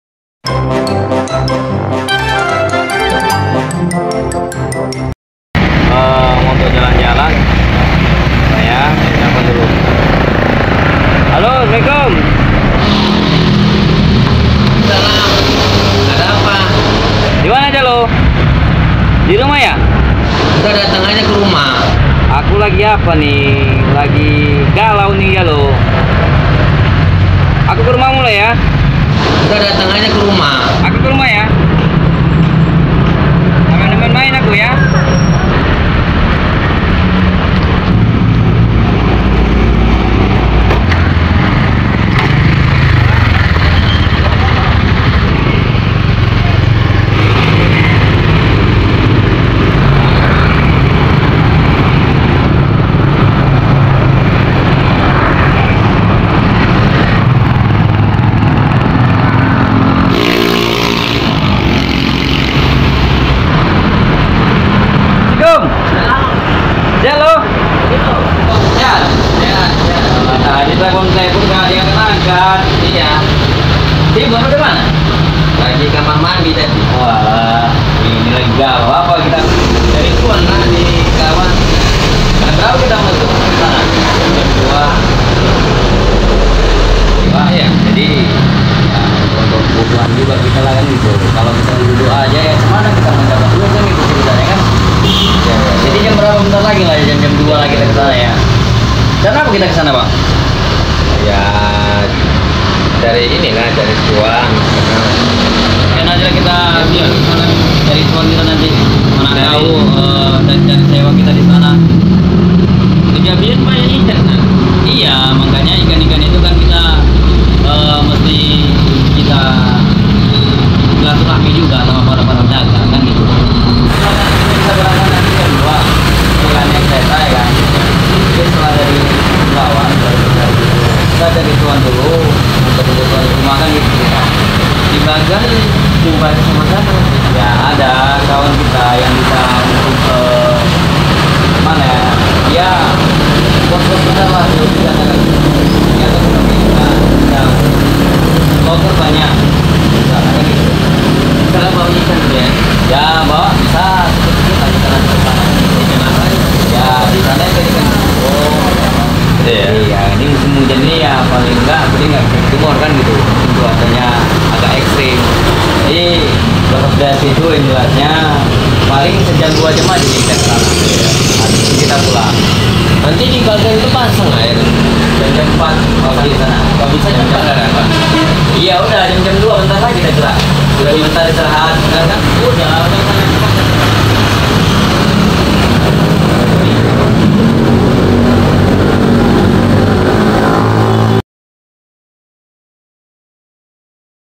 Ngajak Ngopi Sambil Ngobrol Bahas Kerjaan | Warkop Mebel Episode 3
Hello guys jangan lupa Hadir Dukung bersama kami ya

• Pemeran pertama : Zola
• Pemeran kedua : Bj

• Ikutin kami bersama ZTV